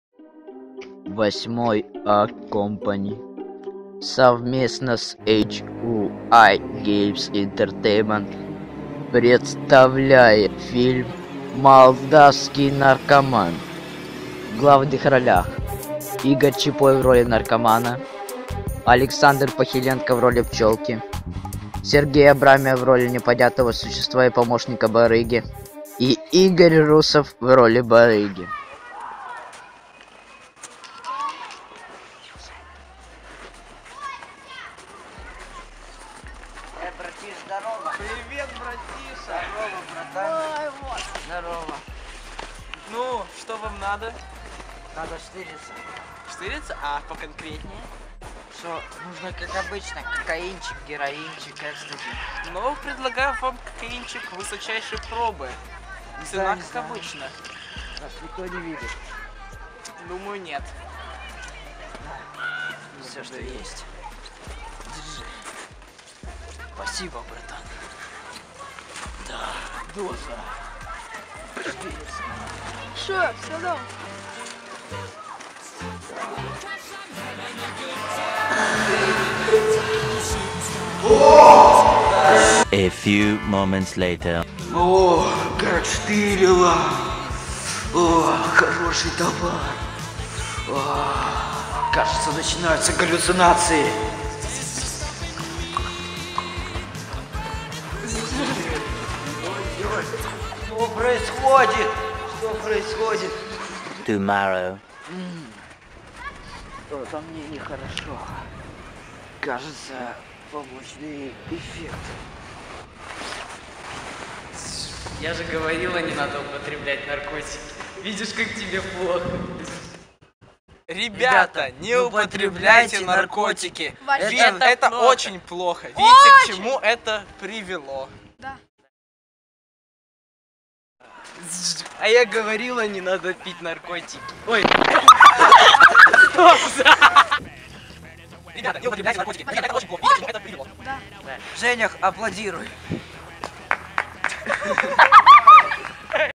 Молдавский наркоман
Наркоман Игорь решил запастить наркотой на целый час вперёд. К сожалению, он попал в передрягу, из которой ему сложно было выбраться. Чем же закончиться судьба нашего героя?
Данное видео изначально было снято для урока по биологии. По стечению обстоятельств, наше видео отклонили ¯\_(ツ)_/¯.